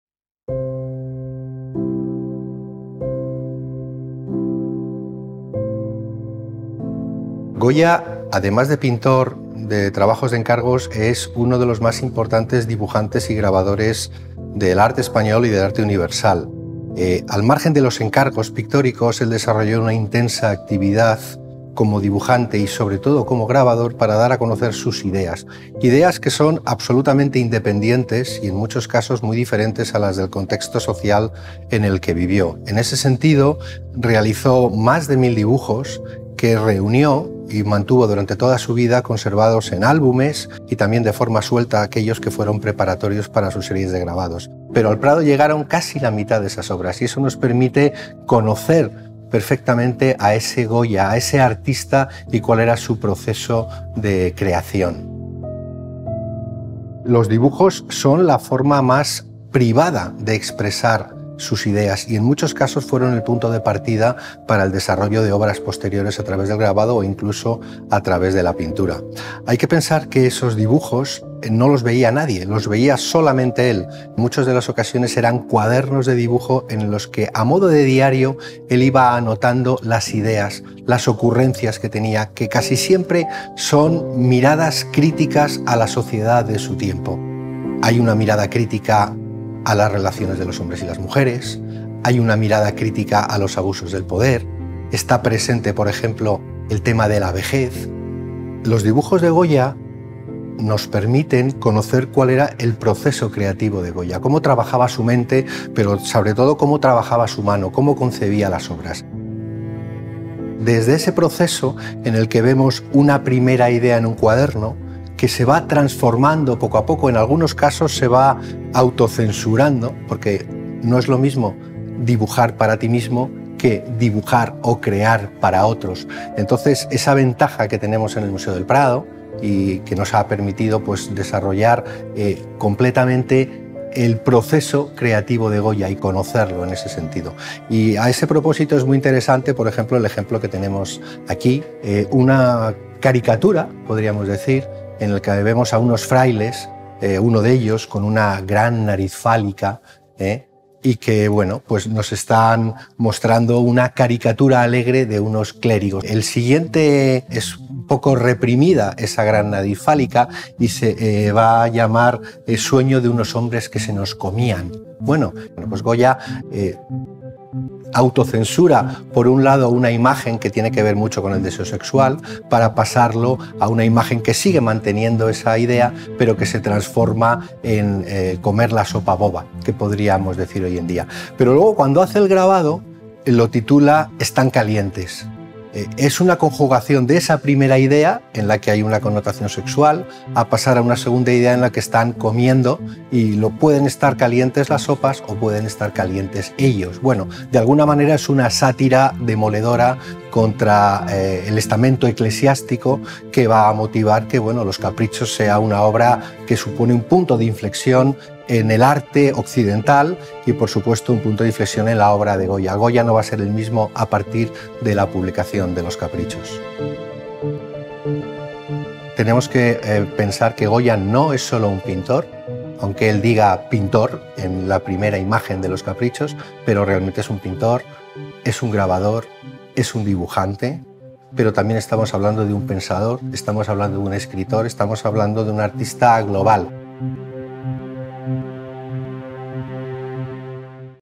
Goya en tu móvil: el Museo del Prado y Telefónica lanzan un curso 'online' sobre el pintor
Se trata de un curso gratuito que consta de cuatro bloques y está enmarcado dentro del programa Prado 'online', que cuenta con el apoyo de Telefónica